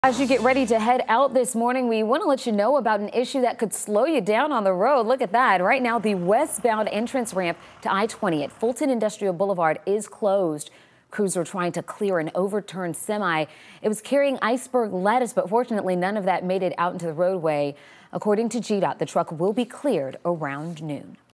Tractor-trailer overturns on ramp to I-20 from Fulton Industrial Blvd.
A tractor-trailer overturned on the ramp to I-20 from Fulton Industrial Boulevard early Sunday morning